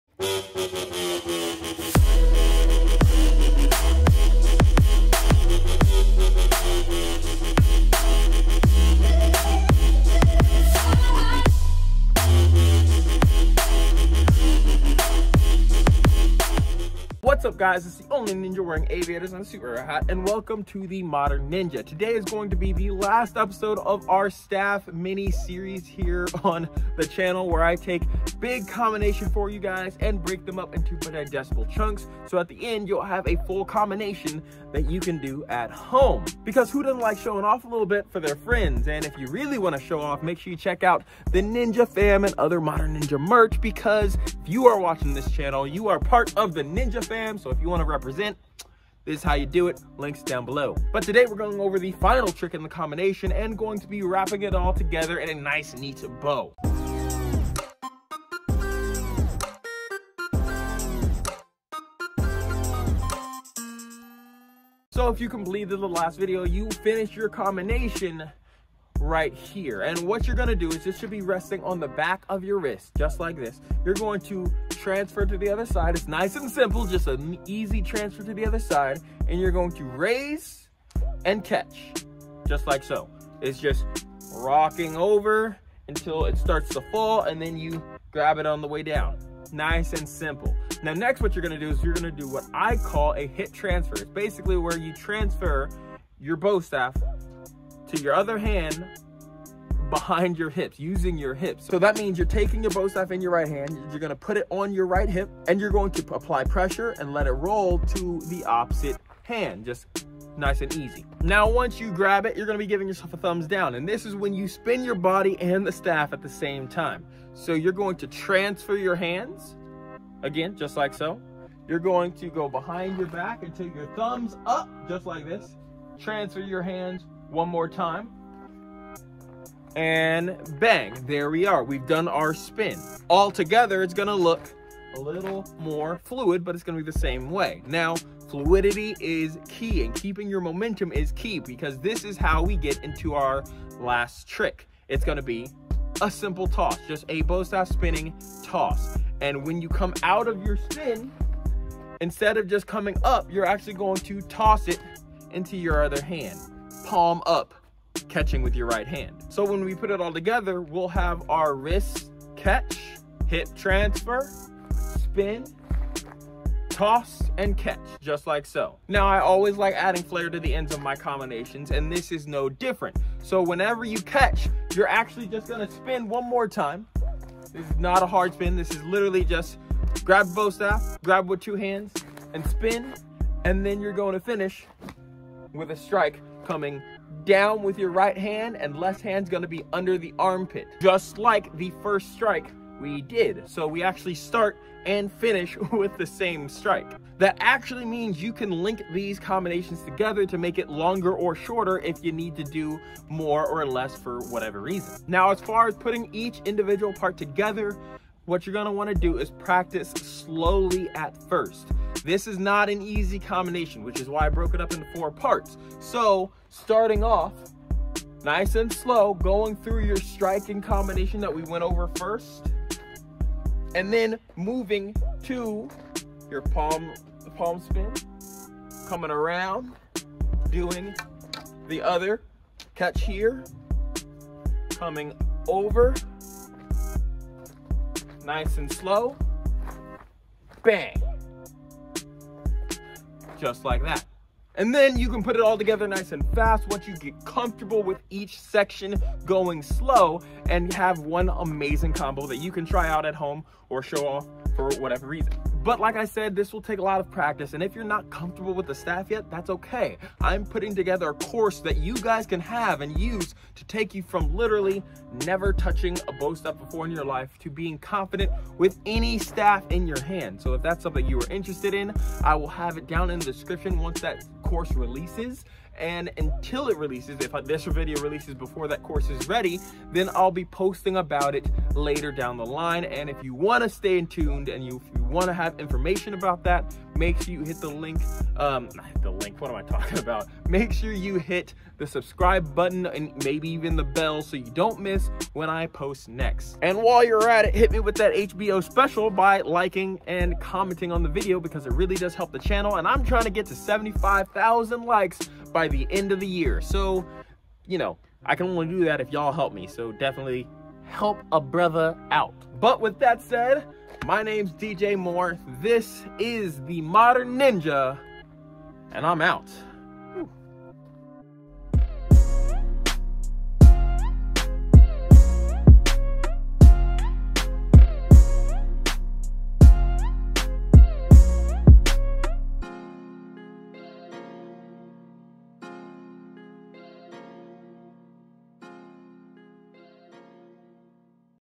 Epic Staff XMA Tricking Combo (4of4) | Martial Arts Tutorial
Ever wonder how to do XMA? Well wonder no more, follow this sereis through the month as I show you how to do this amazing combination!